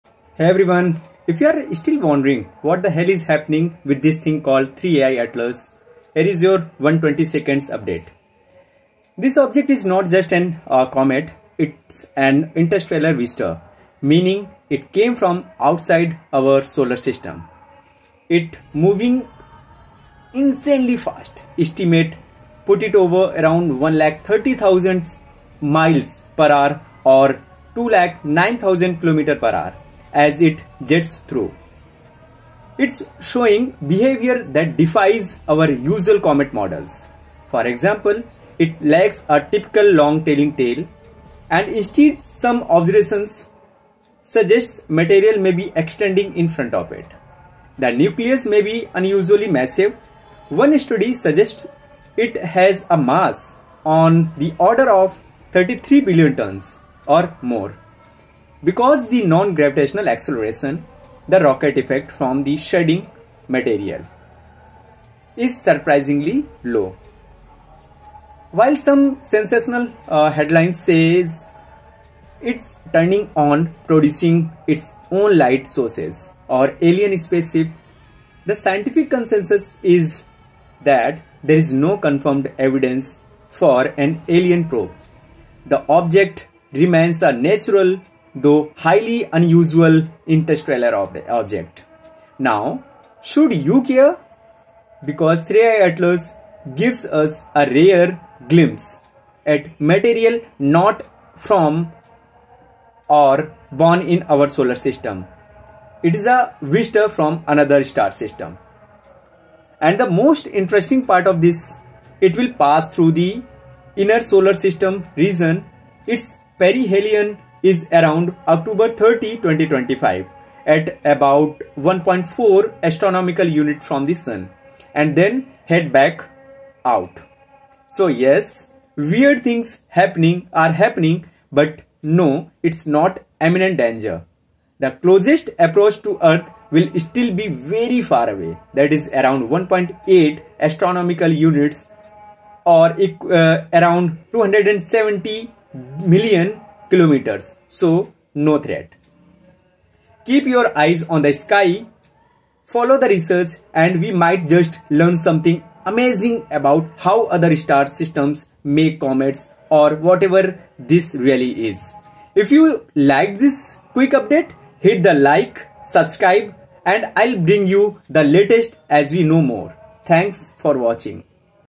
Not a Comet. Not an Asteroid. Then WHAT is 3I Atlas?
A mysterious interstellar object - 3I/ATLAS - is baffling scientists. It’s glowing from within, changing speed, and behaving unlike any known comet or asteroid. 🌠

In this 2-minute update, discover what NASA and ESA have actually confirmed - and what they still can’t explain.